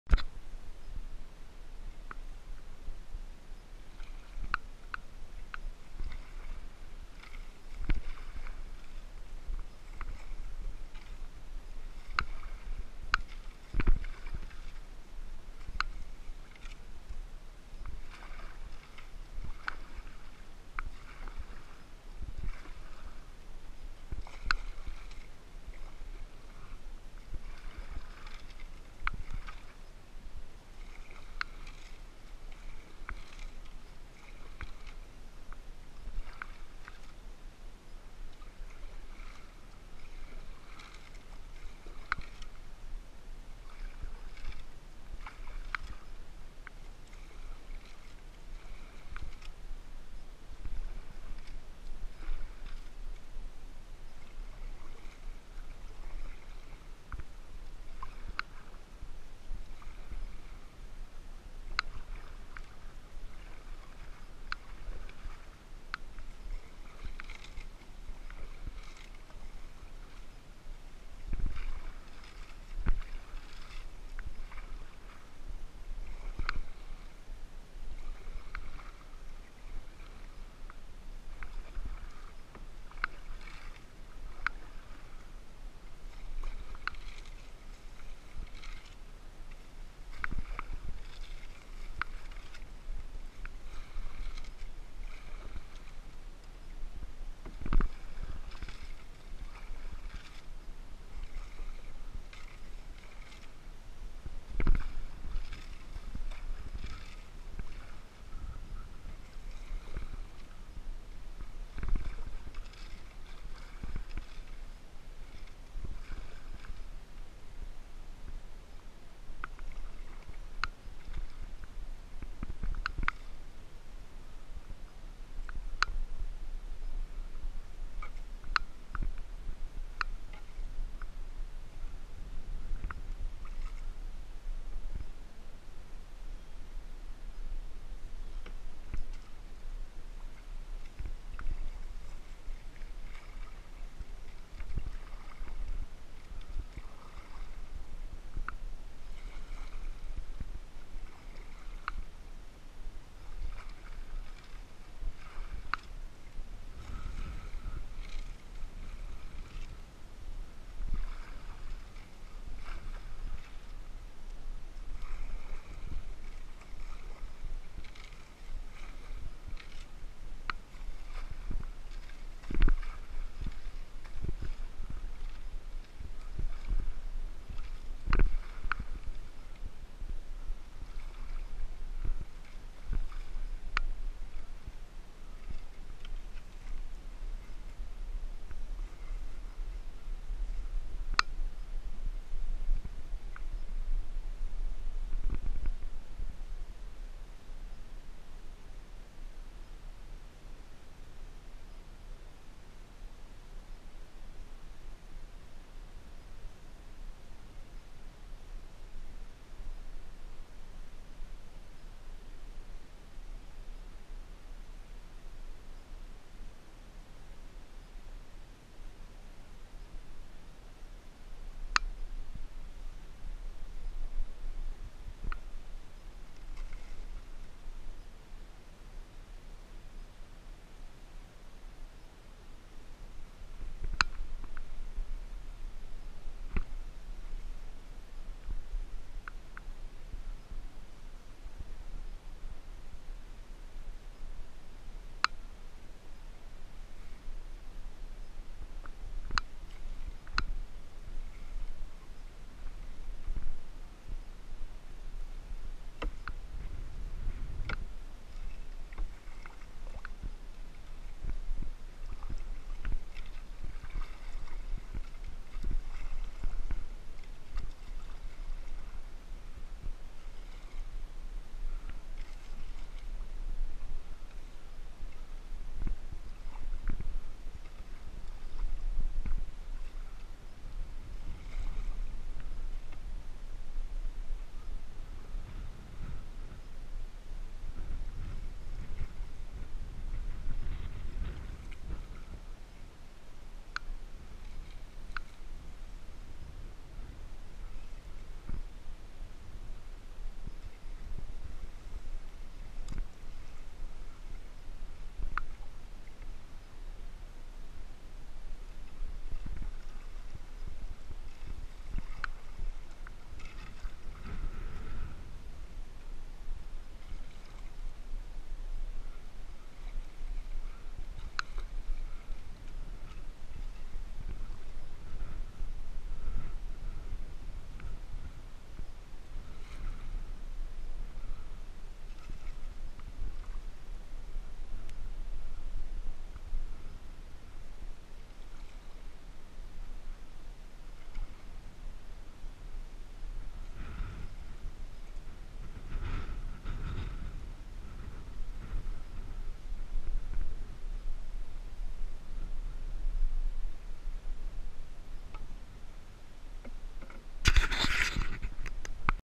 White Feather Lake - Bushwhacking part of the way into this lake from Stuart River in the BWCA
Description:  A bushwhacking paddle along a short creek that runs from White Feather Lake out to the Stuart River through a marsh.  Only made it halfway before the water became too shallow to push a canoe through.  Mud here is deeper than a paddle is long so wading was not an option.  Best to attempt this when water levels are good.  This is in the Sundial Primitive Management Area (PMA) within the BWCA.